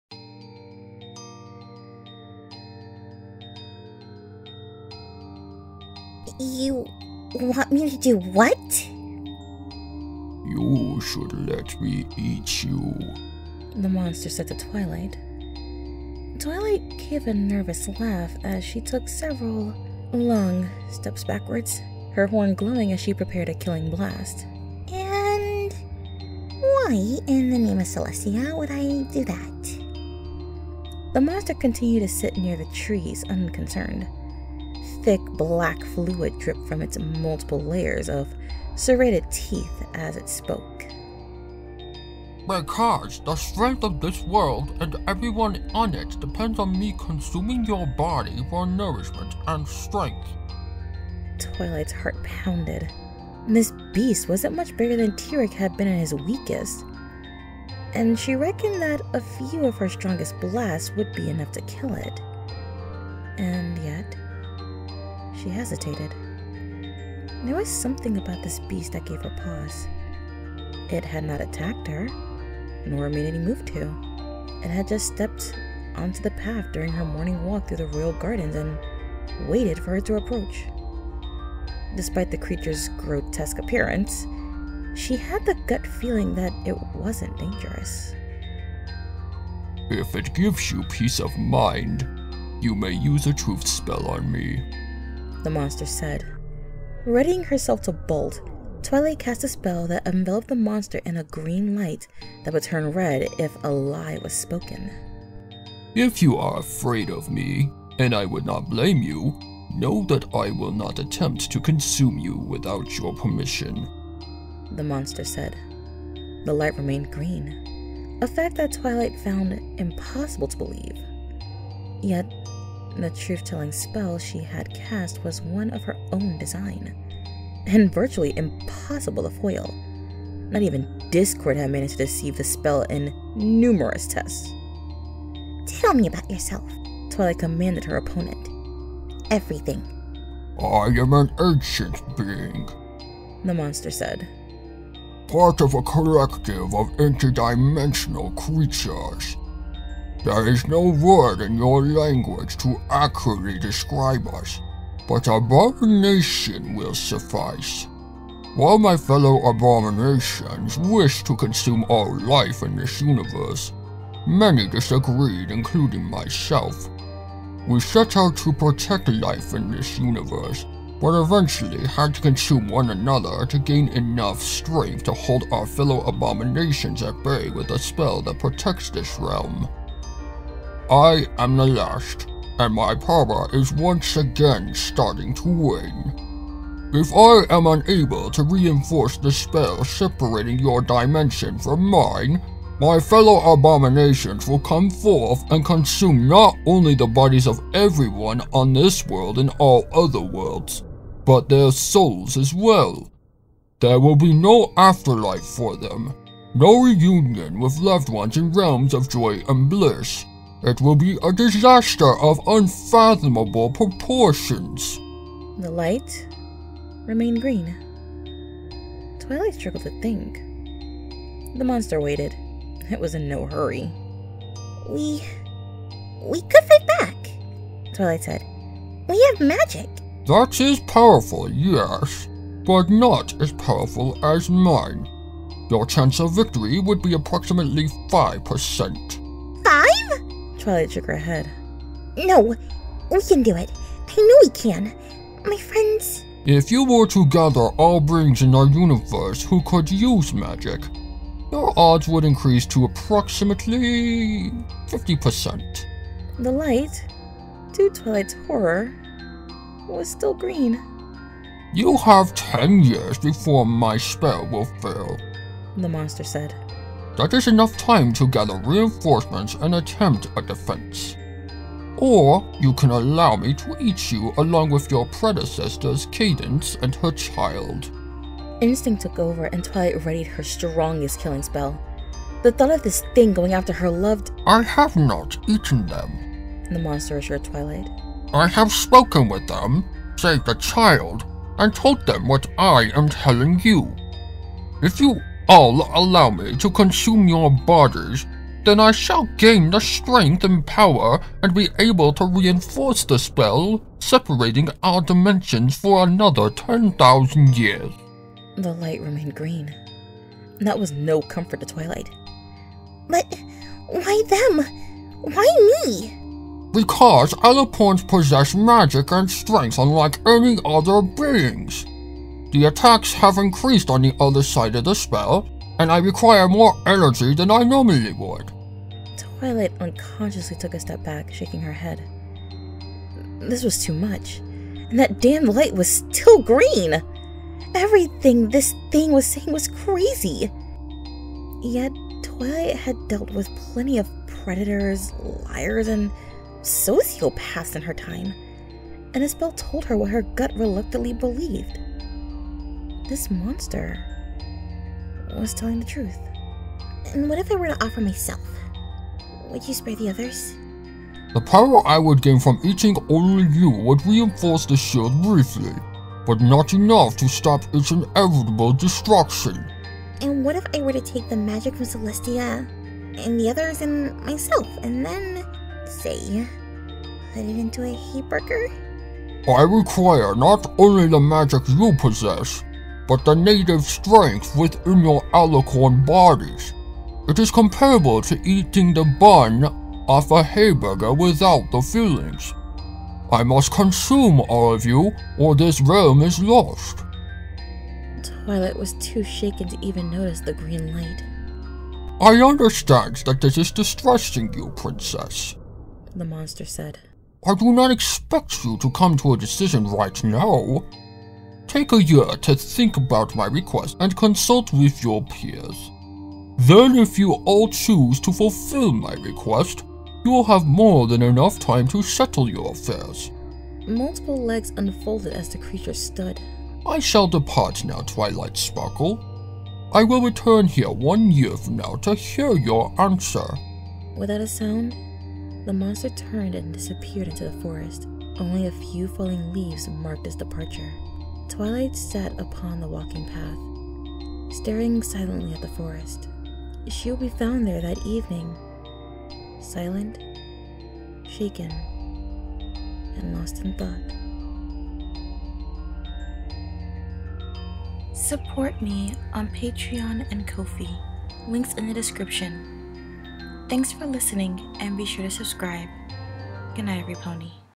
MLP Drama Reading - You Should Let Me Eat You
Twilight Sparkle goes for a walk and is greeted by a monster who asks to eat her.

Perhaps she should let it do so.

Support me!